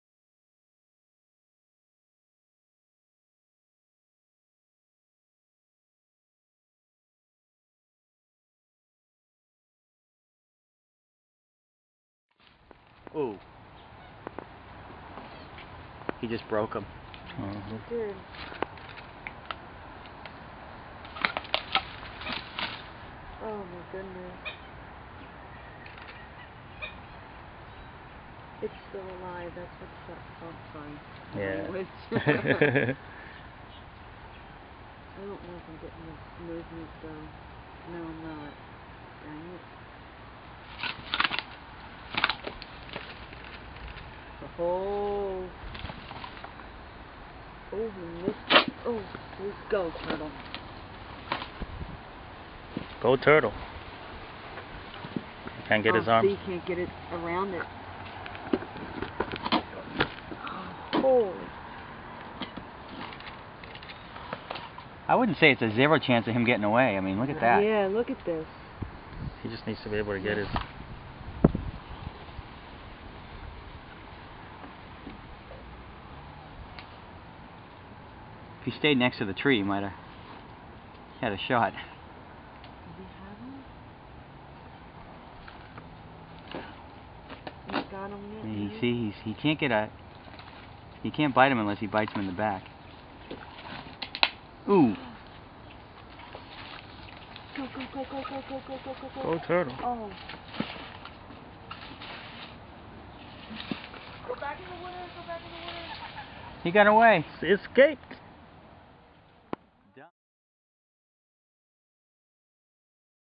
Alligator vs Turtle
One of America's deadliest predators, the American Alligator, is caught in the act trying to make a meal out of an endangered Florida Softshell Turtle